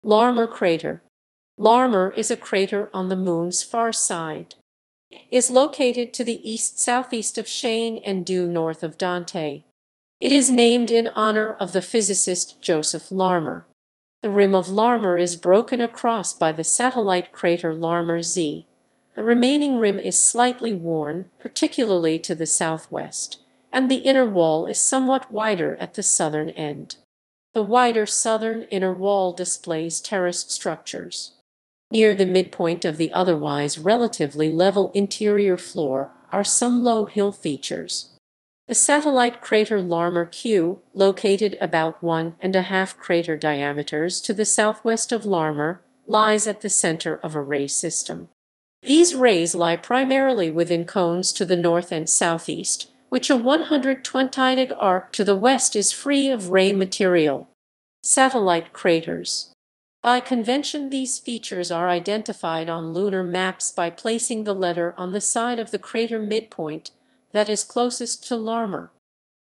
Larmor (crater)
Larmor is a crater on the Moon's far side. It is located to the east-southeast of Shayn and due north of Dante. It is named in honor of the physicist Joseph Larmor.
The rim of Larmor is broken across by the satellite crater Larmor Z. The remaining rim is slightly worn, particularly to the southwest, and the inner wall is somewhat wider at the southern end. The wider southern inner wall displays terrace structures. Near the midpoint of the otherwise relatively level interior floor are some low hill features.
The satellite crater Larmor Q, located about one and a half crater diameters to the southwest of Larmor, lies at the center of a ray system. These rays lie primarily within cones to the north and southeast, which a 120° arc to the west is free of ray material.